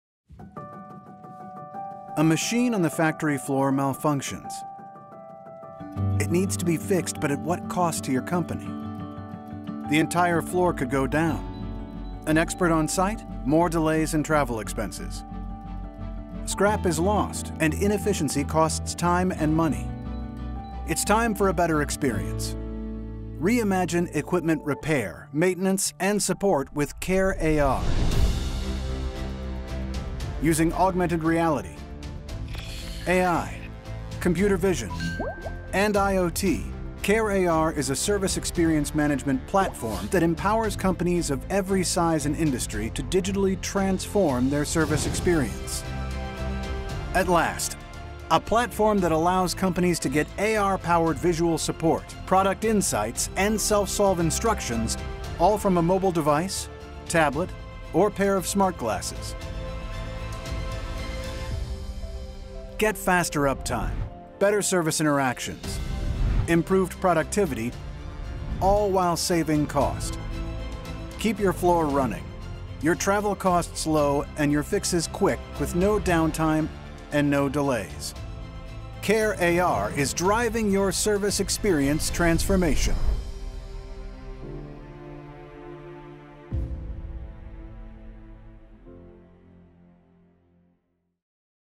CareAR for Manufacturing
A machine goes down on the factory floor. It needs to be fixed but at what cost to your company. Scrap is lost and inefficiency costs time and money. Reimagine equipment repair, maintenance, and support with CareAR. Using augmented reality, AI, computer vision and IoT, CareAR is a visual support platform that empowers companies of every size and industry to digitally transform their service experience.